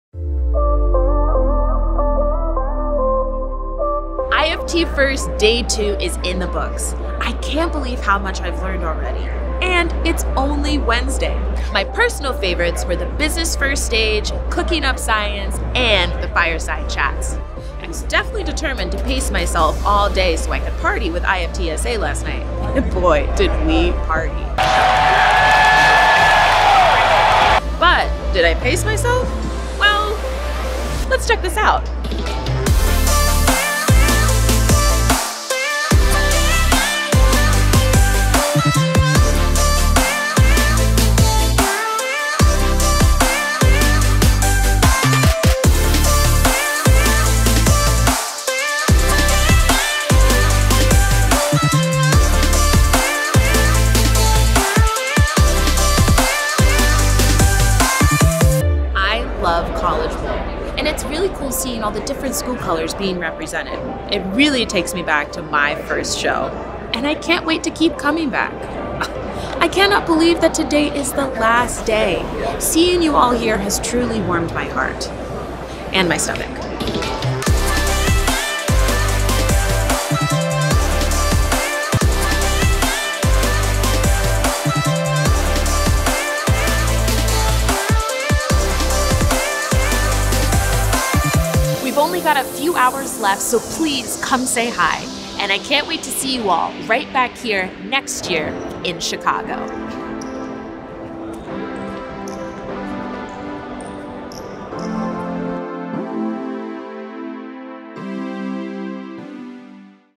IFT FIRST: Day 2
We're wrapping up IFT FIRST today with a full day focused on innovation. Before we get started, let's take a look at all the great stuff that happened here yesterday...and it really was great!